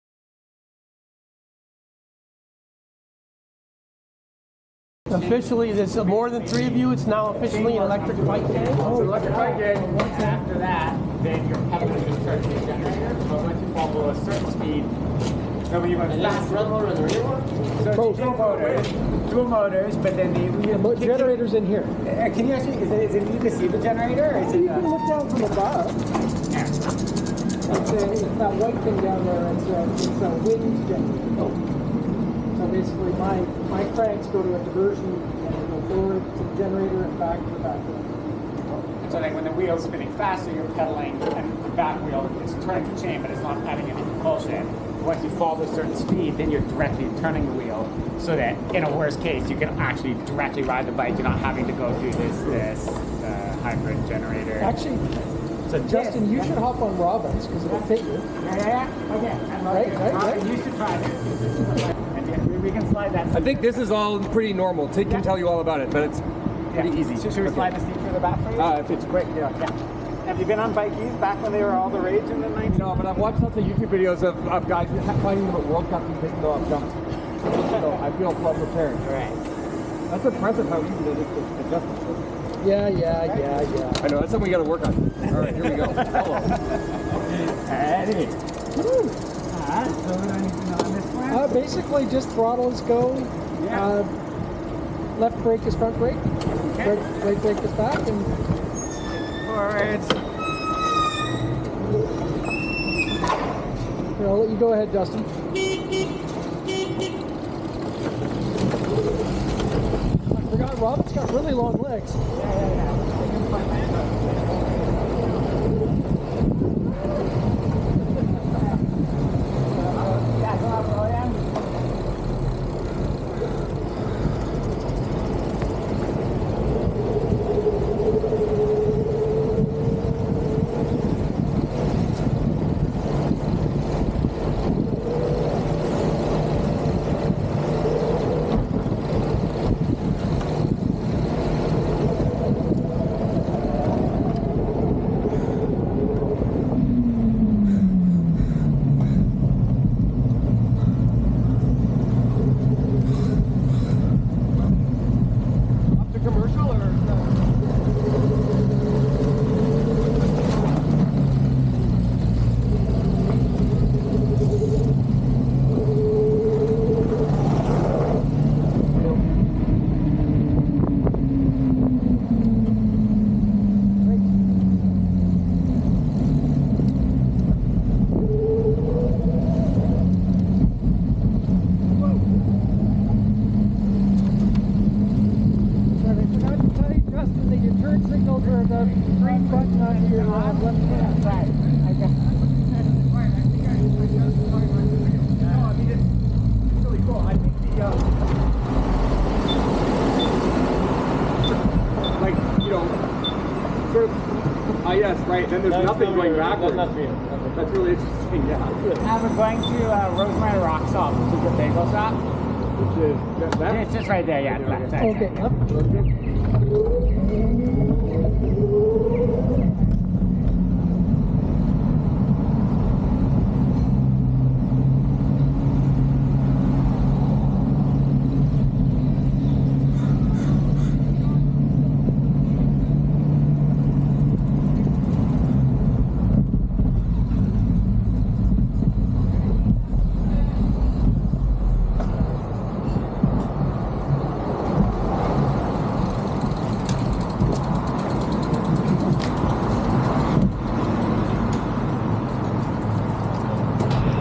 e-cumbent gang
Electrom inventor Fabrizio Cross and engineering advisor Robin Coope recently had the opportunity to drop by Grin Technology for a visit. As luck would have it we bumped into Justin Lemire-Elmore, Grin CEO and chief visionary, and Alon, the inventor of the very exciting new Freegen disk-brake activated regenerative braking hub. We all went for  ride to grab a bagel.